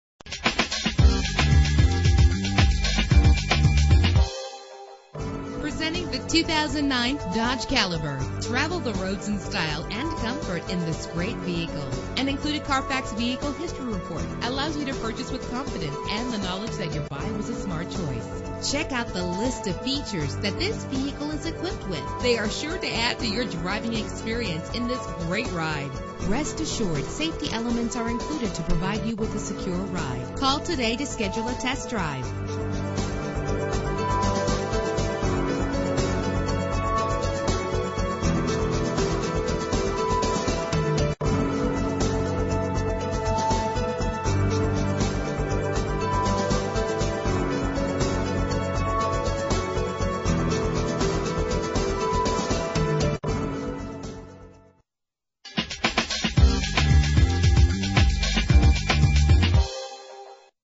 2009 Dodge Caliber - Laurel MD
Year: 2009
Make: Dodge
Model: Caliber
Trim: SXT Sport Wagon 4D
Engine: 4 cylinder 2.0 liter
Transmission:
Color: Copper
Mileage: 47803
Address:
9950 Washington Boulevard